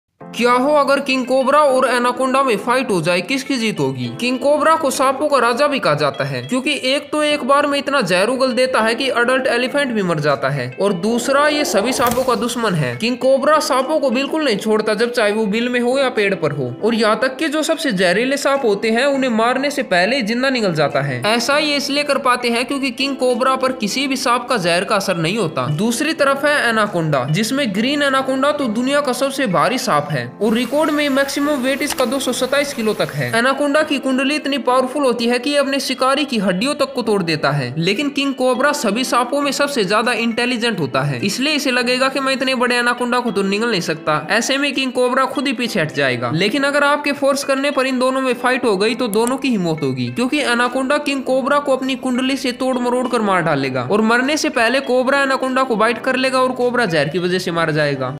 🐍King Cobra vs Anaconda की लड़ाई में किसकी जीत होगी? By Wild Battles
1.This video is not impact on the original (it would positive for all them)
2.This video is all for teaching purpose
3.It is not transformation of nature